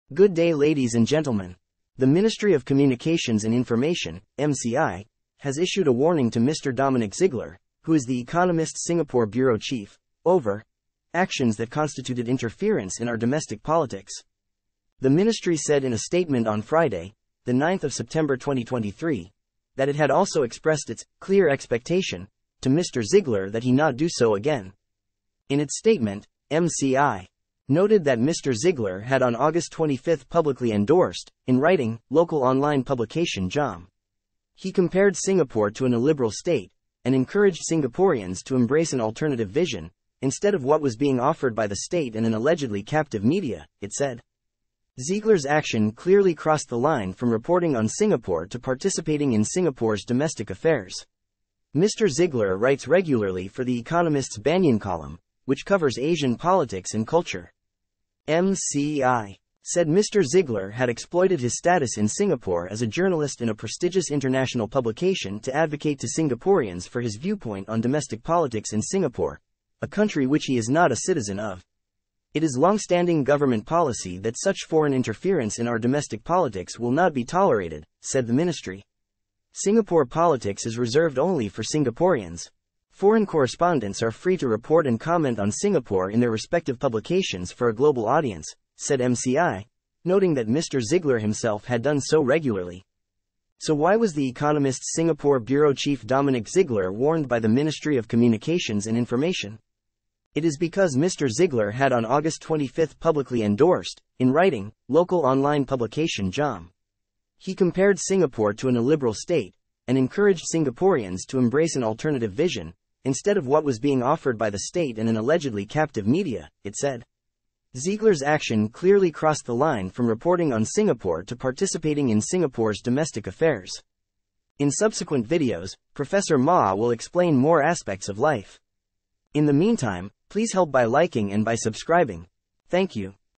Prof Mah Explains Why Ep 86 - Why Was The Economist's Singapore Bureau Chief Warned?
The Ministry of Communications and Information (MCI) has issued a warning to Mr Dominic Ziegler, who is The Economist’s Singapore Bureau Chief, over “actions that constituted interference in our domestic politics”.

The Ministry said in a statement on Friday (9 September 2023) that it had also expressed its “clear expectation” to Mr Ziegler that he not do so again.